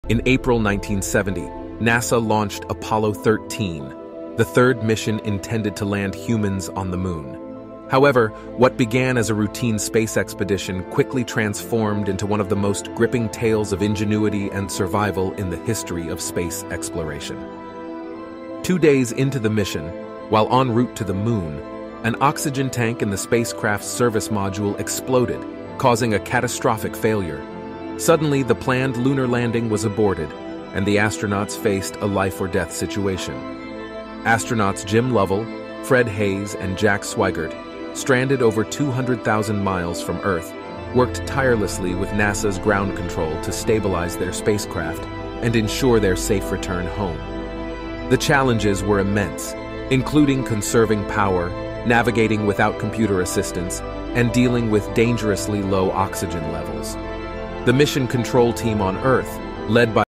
Have You Heard of Apollo 13?
In April 1970, NASA launched Apollo 13, the third mission intended to land humans on the Moon. However, what began as a routine space expedition quickly transformed into one of the most gripping tales of ingenuity and survival in the history of space exploration.

Two days into the mission, while en route to the Moon, an oxygen tank in the spacecraft's Service Module exploded, causing a catastrophic failure. Suddenly, the planned lunar landing was aborted, and the astronauts faced a life-or-death situation.

Astronauts Jim Lovell, Fred Haise, and Jack Swigert, stranded over 200,000 miles from Earth, worked tirelessly with NASA's ground control to stabilize their spacecraft and ensure their safe return home. The challenges were immense, including conserving power, navigating without computer assistance, and dealing with dangerously low oxygen levels.

The mission control team on Earth, led by Flight Director Gene Kranz, worked around the clock to develop solutions for the crippled spacecraft and provide guidance to the astronauts. It was a race against time, as every decision and maneuver had to be precise.

With unparalleled teamwork, the astronauts and mission control overcame one obstacle after another. They managed to use the lunar module, originally intended as their Moon lander, as a "lifeboat" to provide life support and propulsion for their return journey.

The harrowing journey back to Earth was filled with suspense, but against all odds, Apollo 13 re-entered Earth's atmosphere and safely splashed down in the Pacific Ocean. The crew's incredible resilience and the dedication of the mission control team ensured that what could have been a tragic disaster became a story of survival and triumph.

The Apollo 13 mission serves as a testament to human resourcefulness and determination in the face of adversity. It showcases the incredible capabilities of both astronauts and ground control, working together to overcome challenges that seemed insurmountable. It remains a shining example of the indomitable spirit of exploration and the ability of humanity to turn disaster into victory.